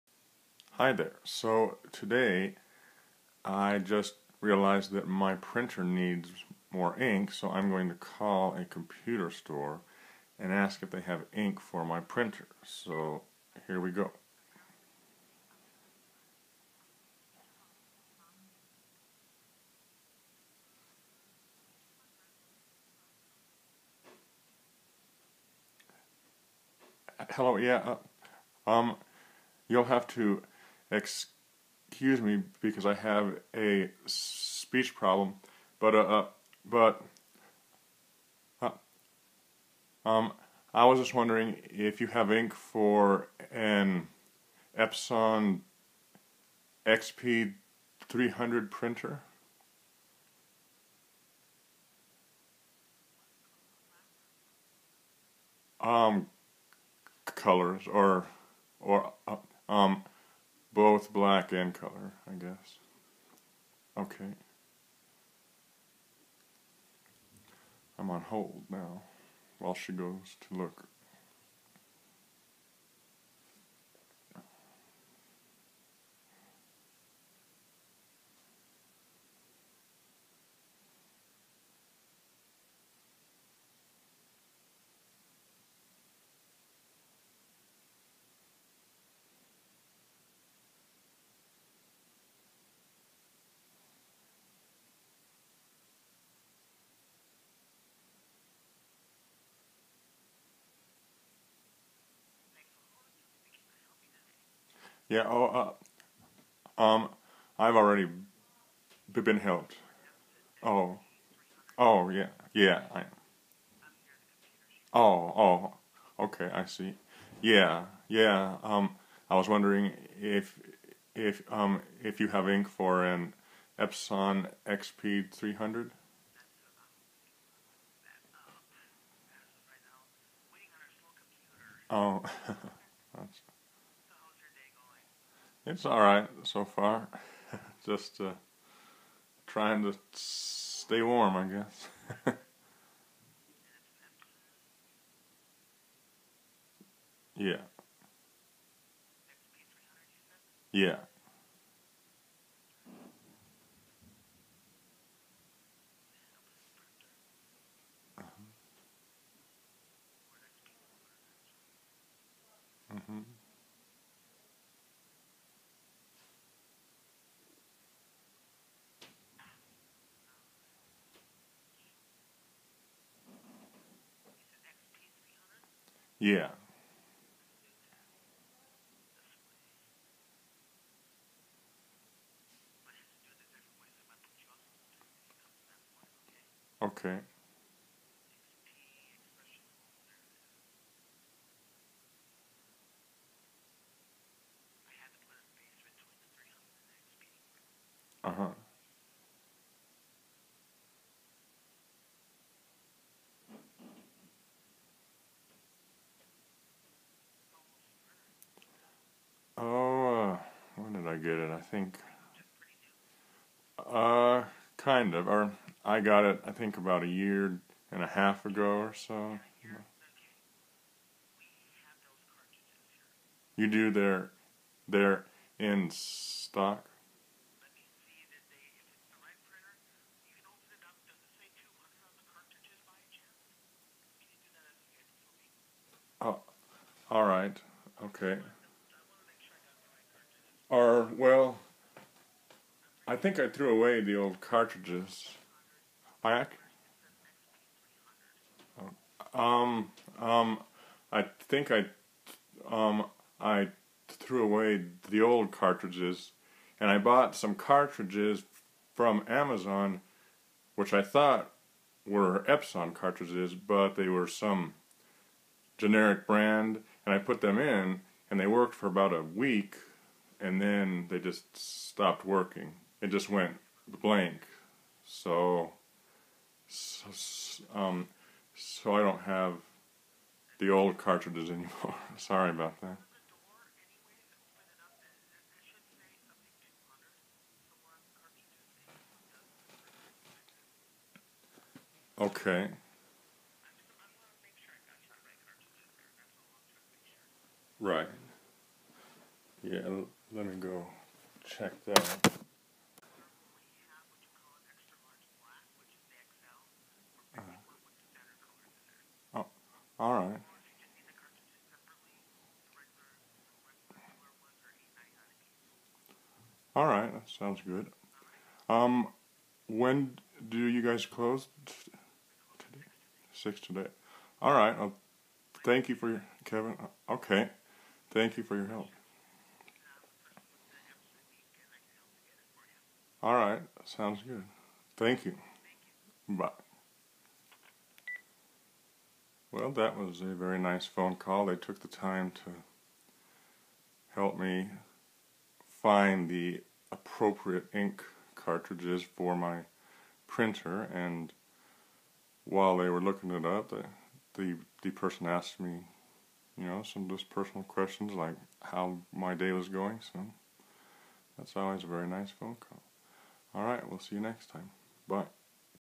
Stutter365 Day 83: Call to Computer Store to Ask About Ink Cartridges
About this video:
My printer ran out of ink so I call a computer store to see if they have the kind of ink I need in stock.